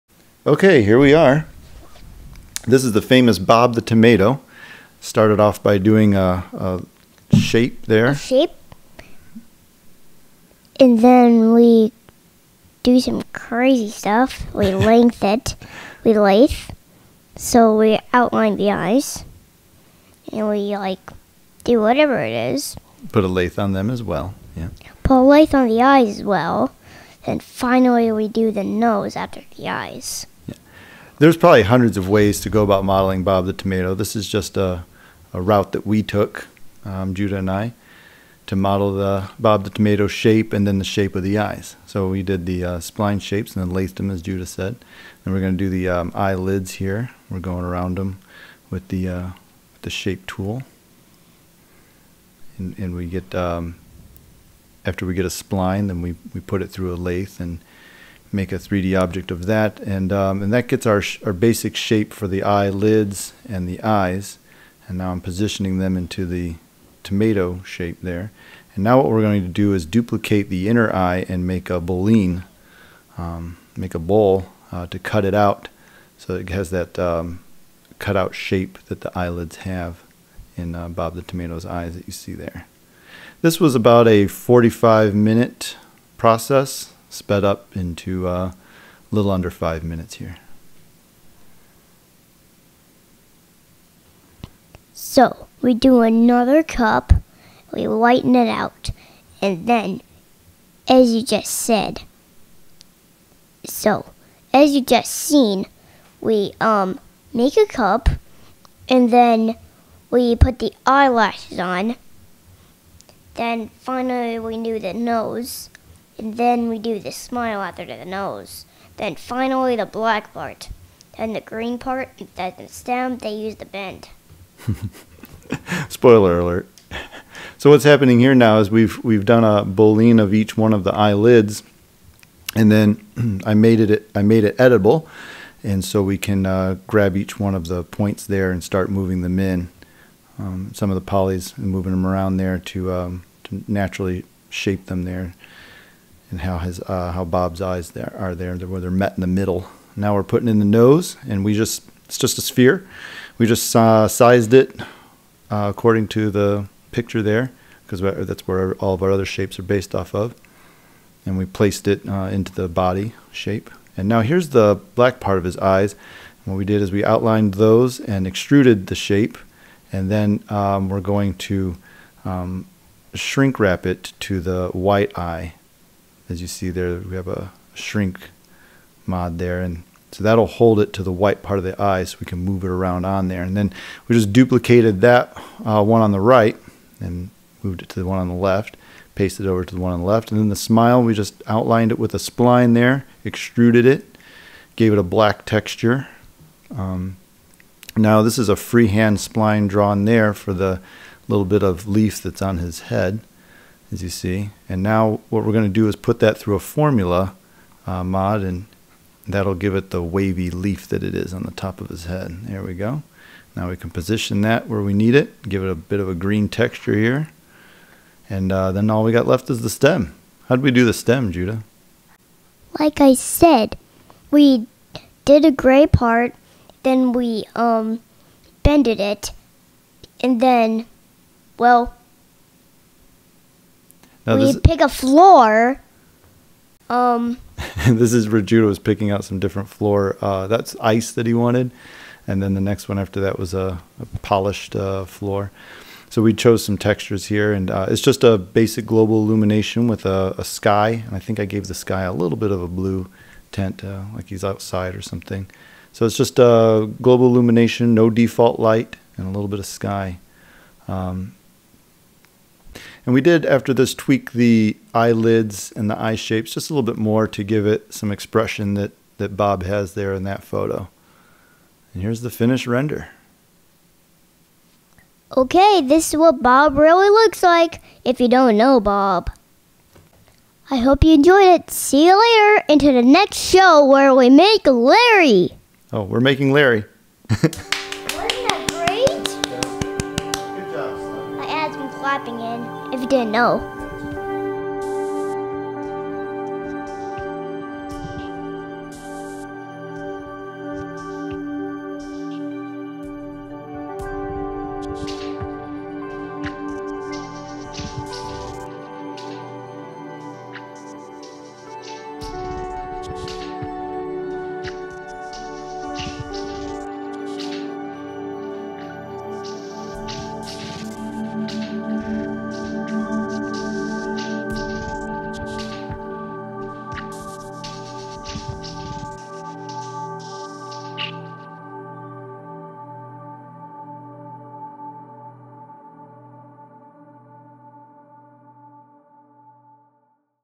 How to Model Bob the Tomato from VeggieTales 3D Character
This is screen capture video of how to model Bob the Tomato from Veggie Tales 3D animation character. This example is shown in Cinema 4d C4d R13 software on mac osx. Total model time was 45 minutes real time, time lapsed to about 5 minutes to see here. Commentary with 3d artist Nathan Goodman and Judah Goodman. Ending music by me :) , Nathan Goodman.